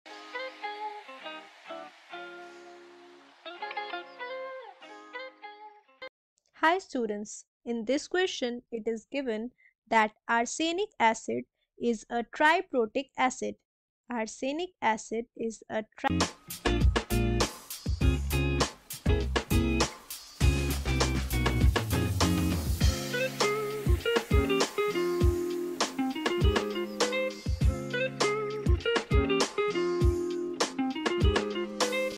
Arsenic acid is a triprotic acid with Ka1=5 x 10^-3, Ka2 = 8 x 10^-8, and Ka3 = 6 x 10^-10. calcula…
Arsenic acid is a triprotic acid with Ka1=5 x 10^-3, Ka2 = 8 x 10^-8, and Ka3 = 6 x 10^-10. calculate [H+], [OH-], [H2AsO4-], [HAsO42-], and [AsO4^3-] in a 0.20M arsenic acid solution.

Join Numerade today at:
DiscoverthePowerofLiquids:BoostYourHealthandWellnessToday!